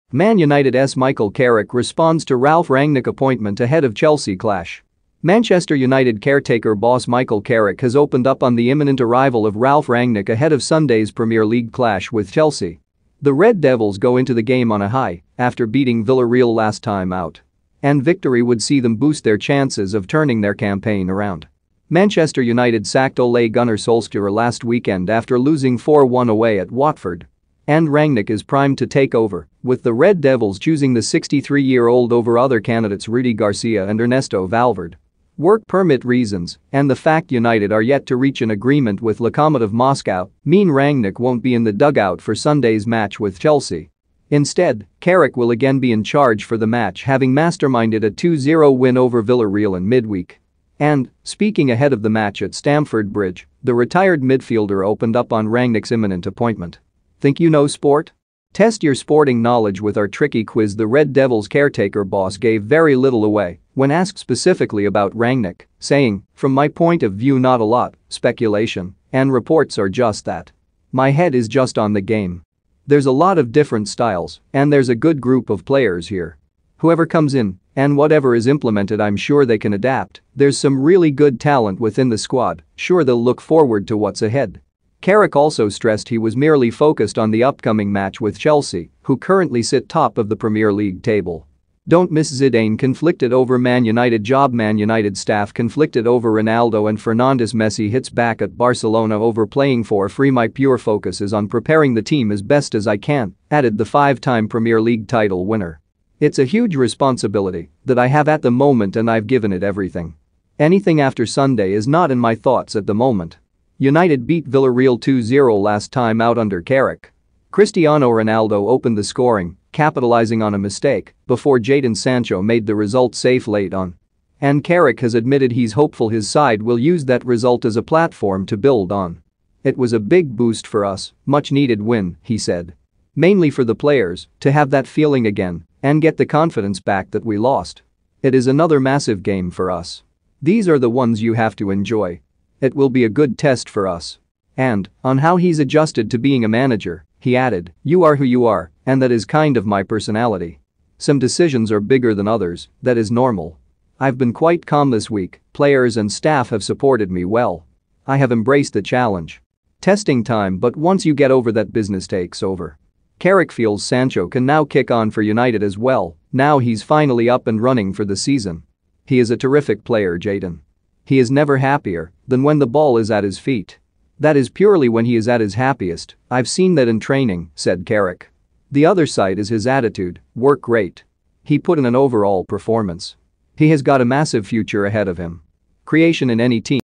Man Utd's Michael Carrick responds to Ralf Rangnick appointment ahead
Man Utd's Michael Carrick responds to Ralf Rangnick appointment ahead of Chelsea clash